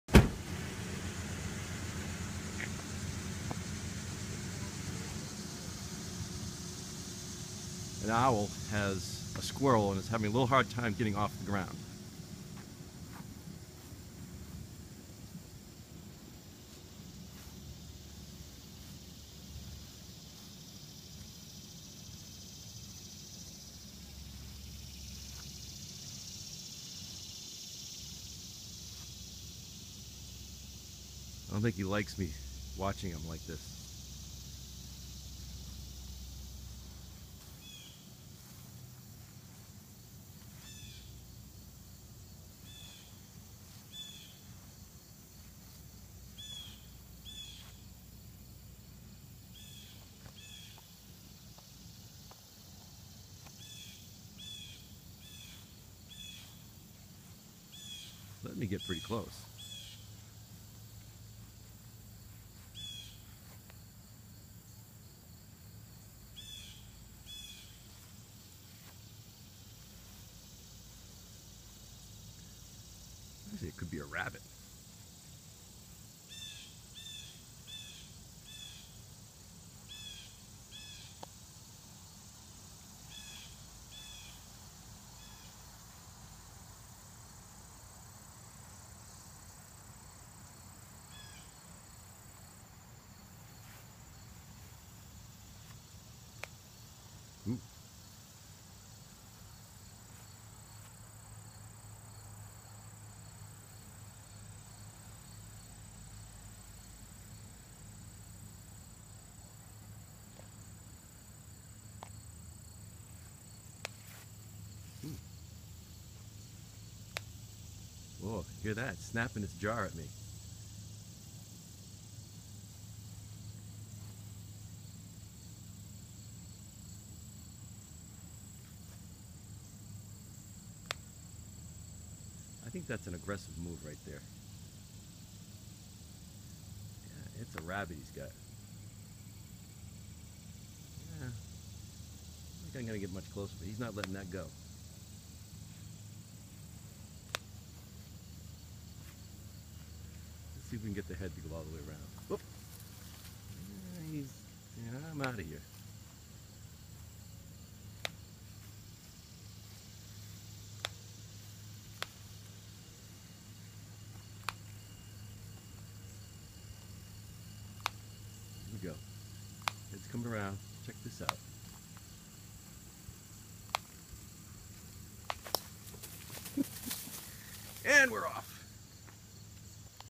Owl tries to fly with heavy rabbit kill
Owl can't get off the ground but protects it's kill